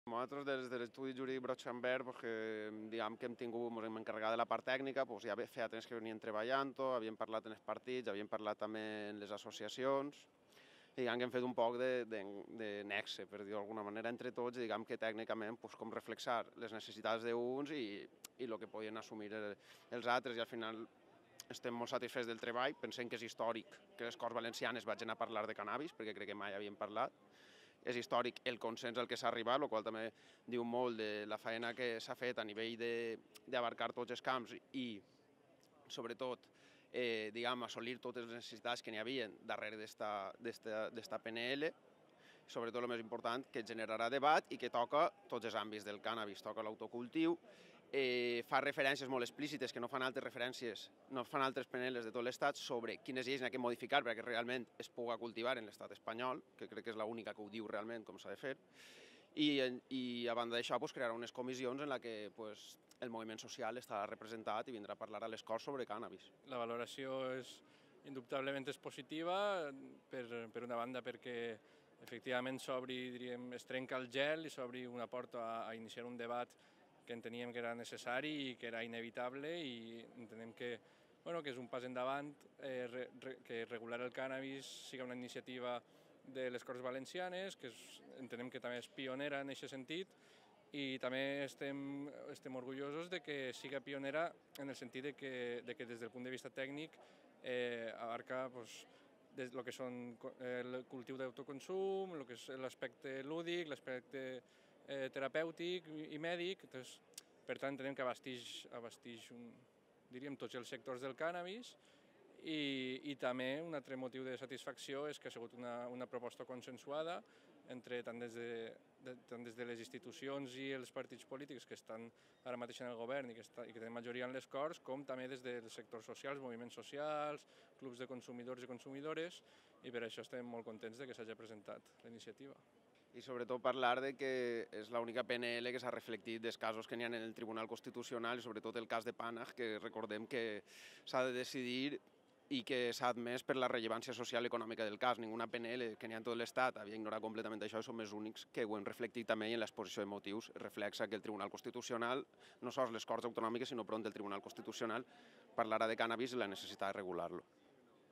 Estudi Juridic Brotsanbert sobre PNL CANNABIS 2017 Cortes Valencianas
Toda la info sobre la PNL Cannabis presentada en las Cortes Valencianas el 16/06/2017

Desde Brotonsandbert se han preocupado de la parte técnica, han hablado con los partidos, con las asociaciones, haciendo un poco de nexo , tratando de reflejar las necesidades de unos satisfechos del trabajo, es histórico que las cortes valencianas vayan a hablar de cannabis, es histórico el consenso que se ha obtenido a nivel de abarcar todos los campos y sobretodo
toca todos los aspectos del cannabis, el autocultivo,  hace referencias que leyes del estado español habría que modificar para que realmente se pueda cultivar en el Estado Español,
Nuestra valoración es positiva sobretodo porque se rompe el hielo y se trata una cosa tan necesaria y que era inevitable, y es un paso adelante, que regular el cannabis sea una iniciativa de Las Cortes Valencianas, que entendemos que también es pionera en este sentido y nos sentimos orgullosos de que desde el punto de vista técnico abarca desde el cultivo de Autoconsumo, el aspecto lúdico, el aspecto terapéutico y médico pues abastece todos los sectores del cannabis y otro motivo de satisfacción es que ha sido una propuesta consensuada entre las instituciones y los partidos políticos que ahora mismo están en el gobierno y tienen mayoría en las Cortes como de los movimiento social, clubs de consumidores y consumidoras y por eso estamos muy contentos de que se haya presentado esta iniciativa y sobretodo hablar de que es la única PNL que habla de los casos que están en el Tribunal Constitucional, como el caso de Pannagh que recordemos se tiene que decidir y que se ha admitido por la relevancia social y económica del caso. Ninguna PNL lo ha destacado u somos los únicos que lo hemos reflejado también en la exposición de motivos, por lo que no sólo las Cortes sino que el Tribunal Constitucional pronto hablará también de cannabis y la necesidad de regularlo.